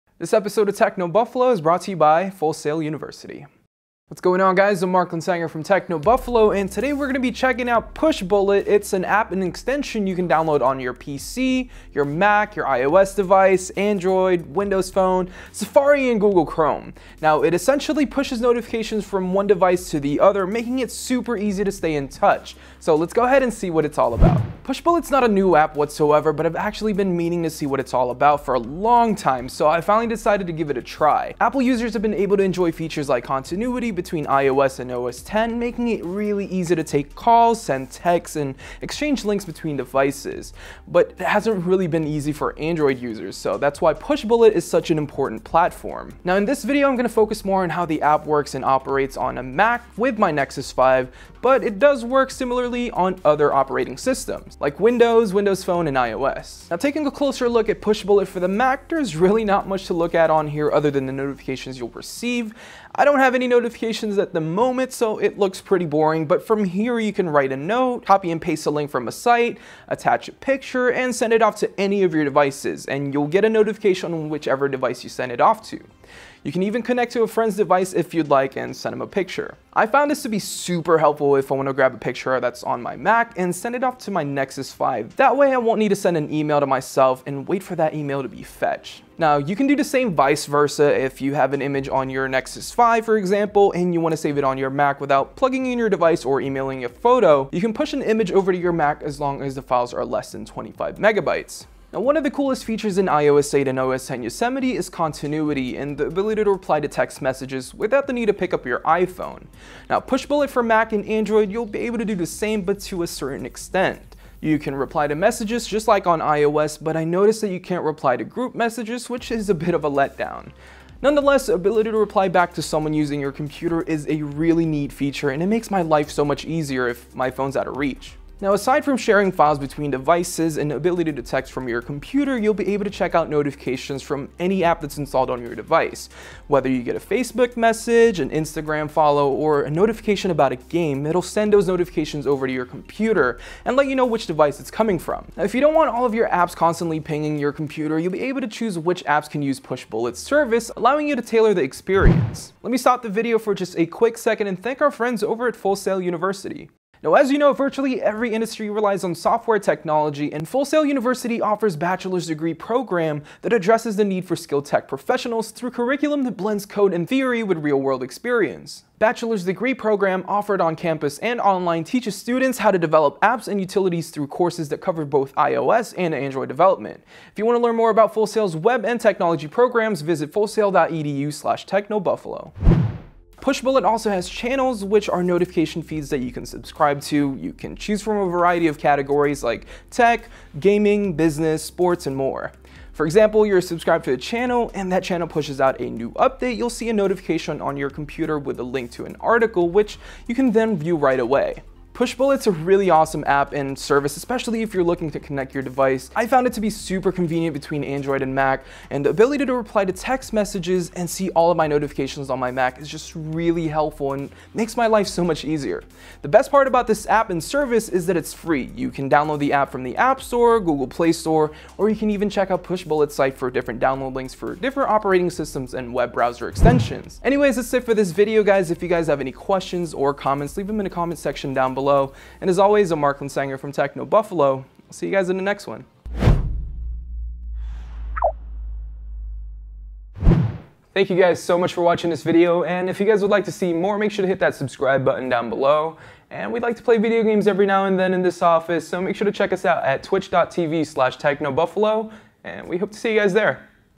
Pushbullet - Better Than Continuity?
You’ve probably heard of Pushbullet—the app is famous as a solution for getting mobile notifications to your computer, or visa versa. It’s dead simple to use, and available for pretty much every platform out there (mobile, desktop and browser). And best of all, it’s free. You have no reason not to try it out.